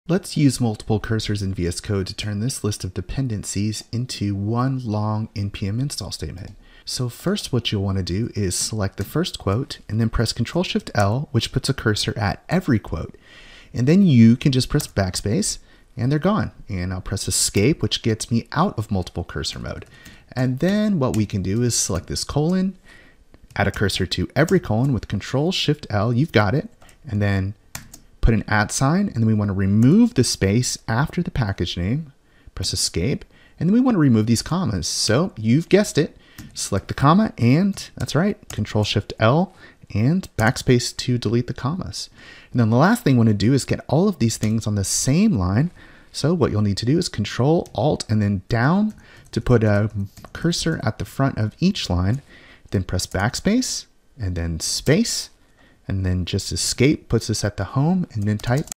How to use multiple cursors in VS Code
Multiple cursors is a powerful way to edit text, but the concept is a bit intimidating. Once you see them in action, they make much more sense!